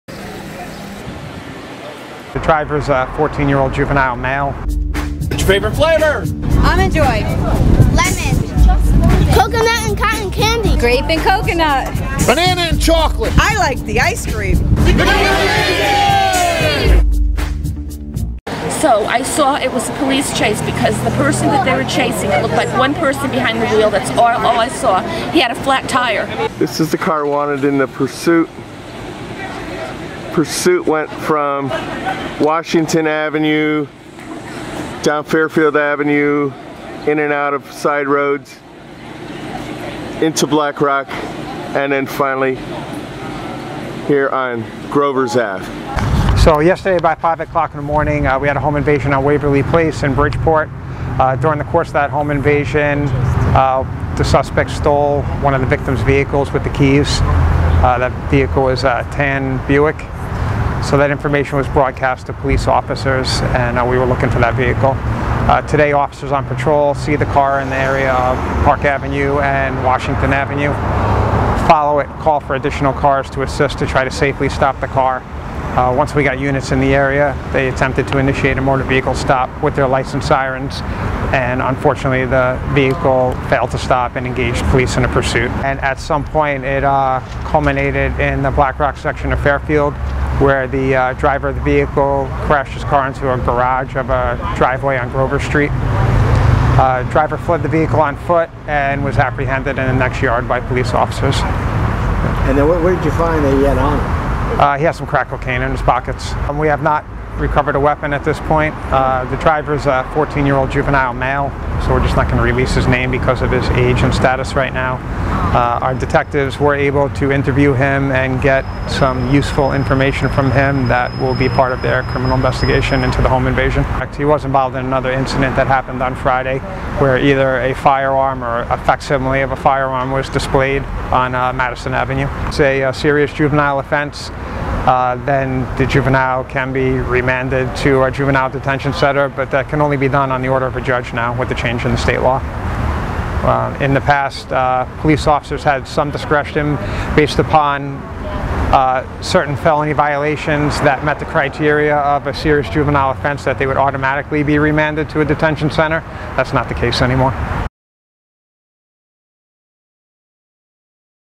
Bridgeport News: Pursuit of 14 Year Old In Home Invasion & Stolen Vehicle
Bridgeport CT-- Today police located a tan Buick that was stolen with keys from Waverly Place overnight during a home invasion.  Police this morning located the vehicle at Park and Washington Avenue.  Police coordinated additional police cars in an attempt to stop safely the vehicle but the vehicle took off on them.  The vehicle ran through the Hollow section of town to downtown to Black Rock where the pursuit came to an end on Grovers Avenue.  The suspect, a fourteen-year-old boy had hit a garage and then fled on foot.  Police had quickly apprehended the juvenile in a backyard.  No weapon was located but he had crack cocaine in his pockets. The boy's mother said he was taught to drive at age ten by his father who is a mechanic.  Captain Fitzgerald said the boy attempted a robbery with a firearm, or a facsimile firearm, on Friday on Madison Avenue.  The boy's mother said off camera that "he was remorseful and I have to hope he’s learning from this to stop following the leader.”